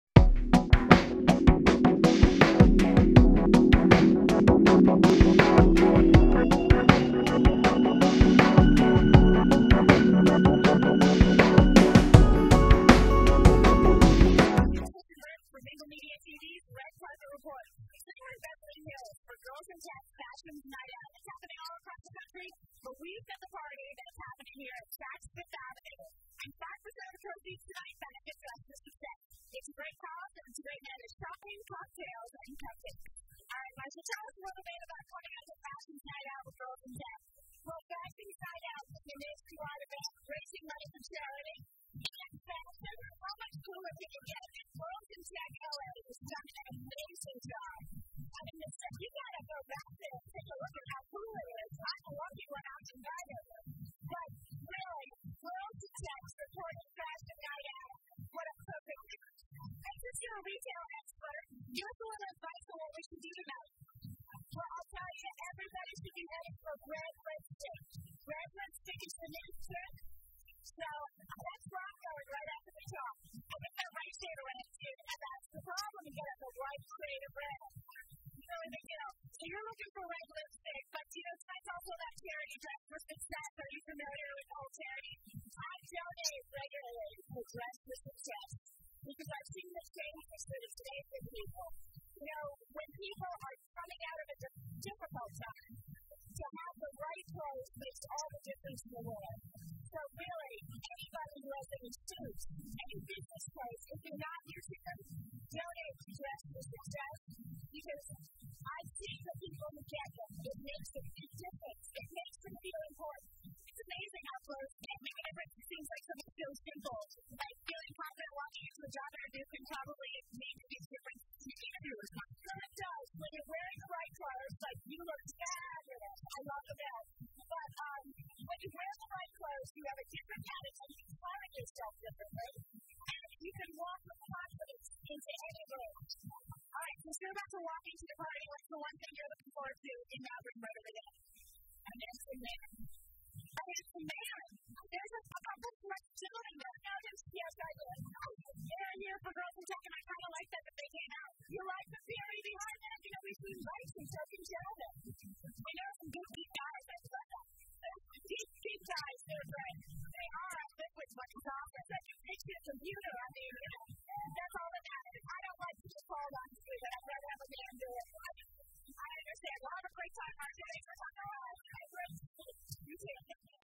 Marsha Collier at Fashion's Night Out 2010 @Saks Beverly Hills
Girl?s In Tech Los Angeles and A Gadsby Affair created the Beverly Hills event with the glitz and glamour expected at any red carpet event and will have media coverage by online media and live web TV network company, Mingle Media TV, who was their with correspondent, Kristyn Burtt, capturing the event on video to share on their Red Carpet Report web show. To follow in the theme of ?Fashion?s Night Out? of shopping and giving, Girls in Tech Los Angeles selected the non-profit, Dress for Success to support. Giving back to the community is a big part of what GITLA does in organizing events like this one.